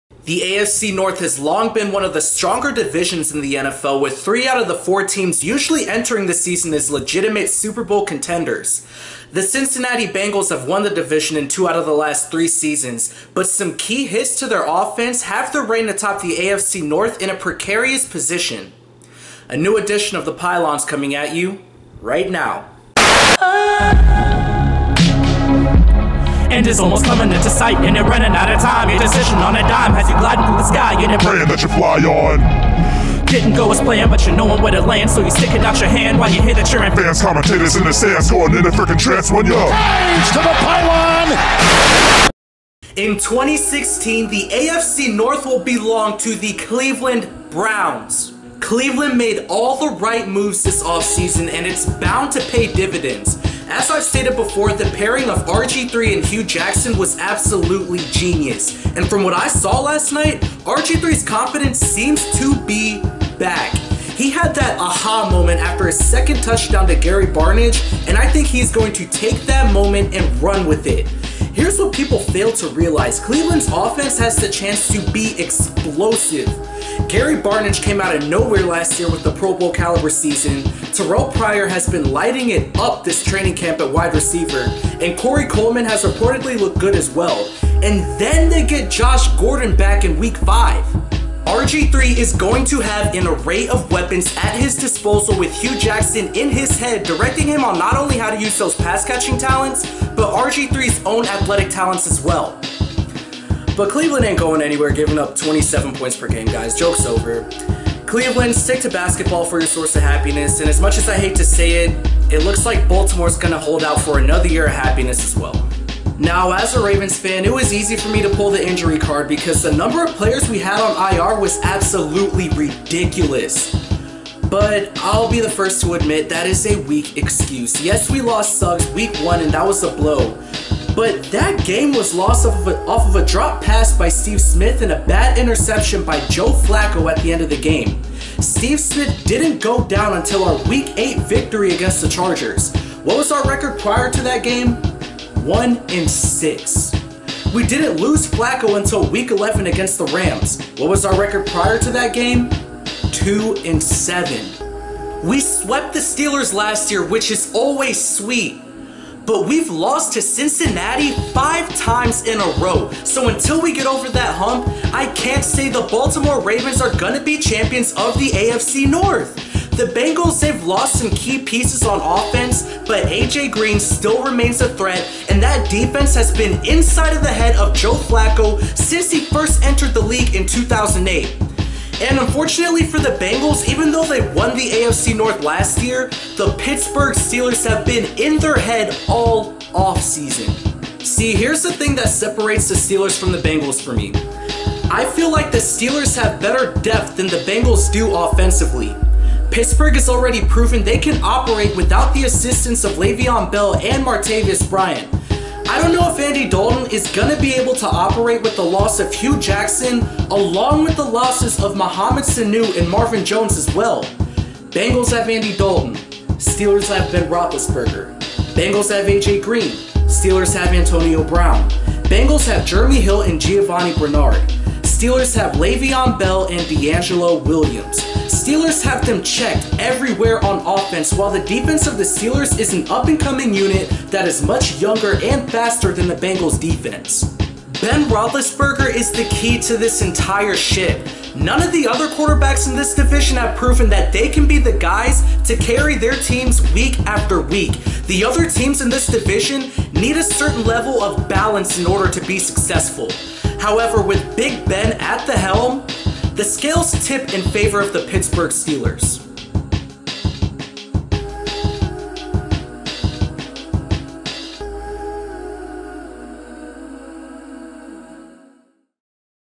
Who Will Win the AFC North?
The AFC North is always a competitive division and this season will be no different. The Cincinnati Bengals finished with a 6-2 division record last year while winning the North for the second time in the past three years. Will they be able to hold onto the AFC North crown in 2016?